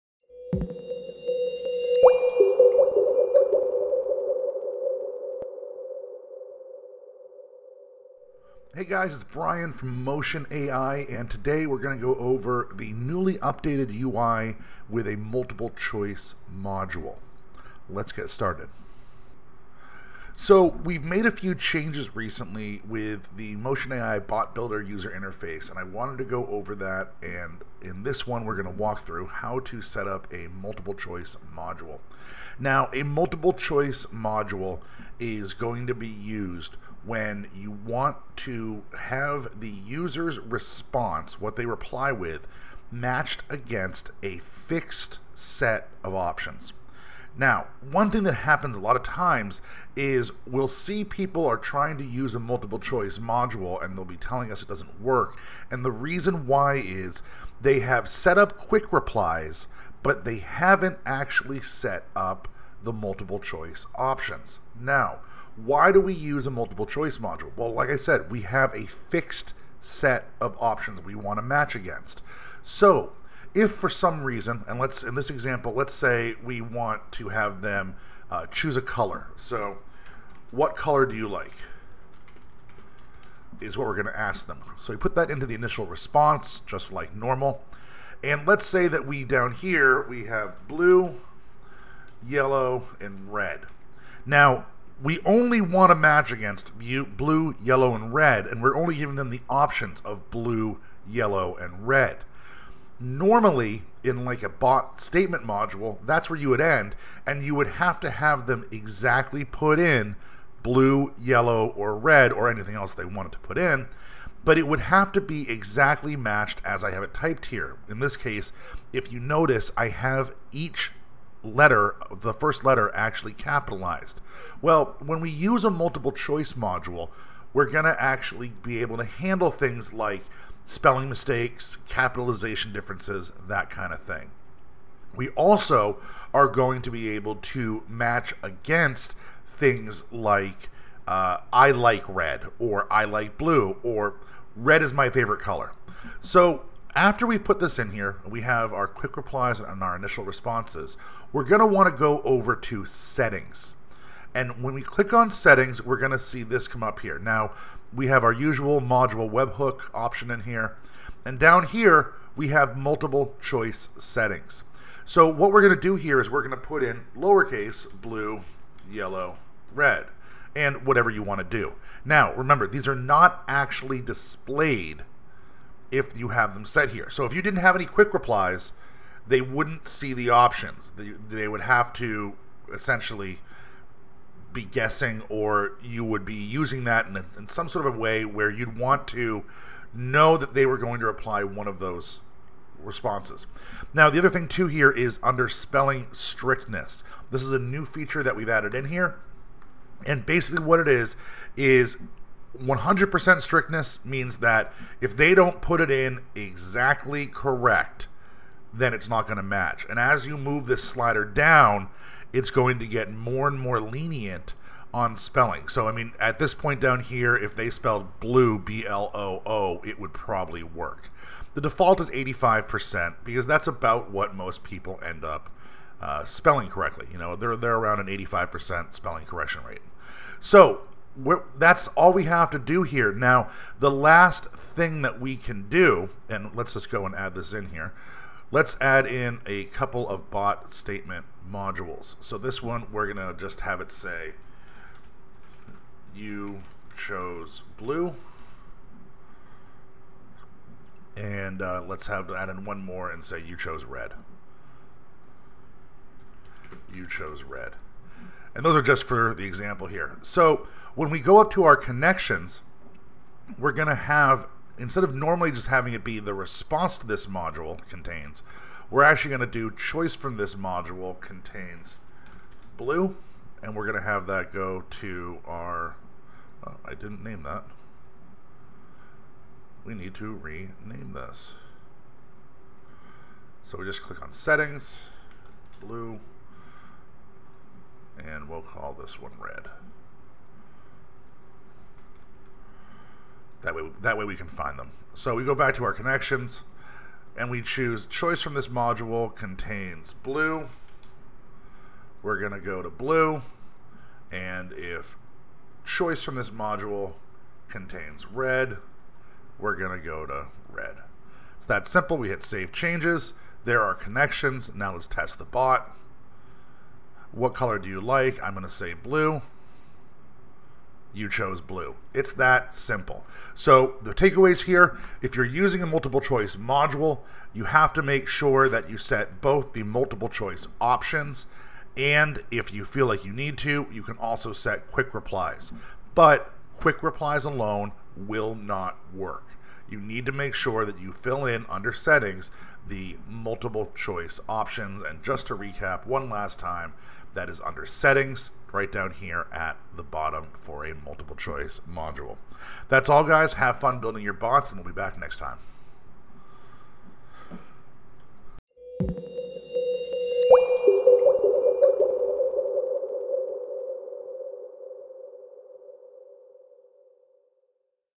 Motion.AI Multiple Choice Module Tutorial
A quick walkthrough and tutorial on how to use the "Multiple Choice Module" in the Motion.AI Visual Bot Builder